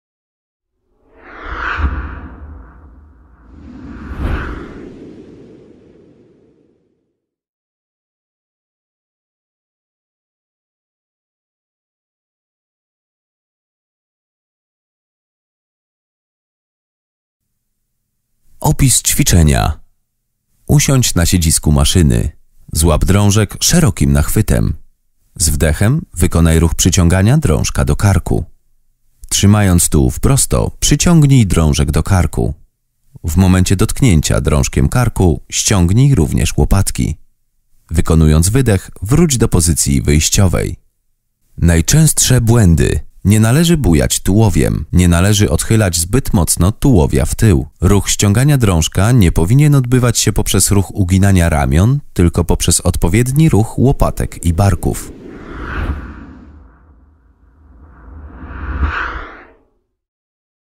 Trening pleców - ściąganie drążka wyciągu górnego do karku - Atlas Ćwiczeń SFD
Usiądź na siedzisku maszyny. Złap drążek szerokim nachwytem. Z wdechem wykonaj ruch przyciągania drążka do karku. Trzymając tułów prosto, przyciągnij drążek do karku. W momencie dotkniecia drążka karku ściągnij również łopatki. Wykonując wydech wróć do pozycji wyjściowej.

Uwagi:
Nie prostuj łokci do samego końca w końowej fazie ruchu. Łokcie powinny być prowadzone wzdłuż bocznej lini ciała. Jest to ćwiczenie, które w znaczącym stopniu obiąża staw ramienny. Dlatego osobom z problemami tego stawu ćwiczenie nie jest polecane.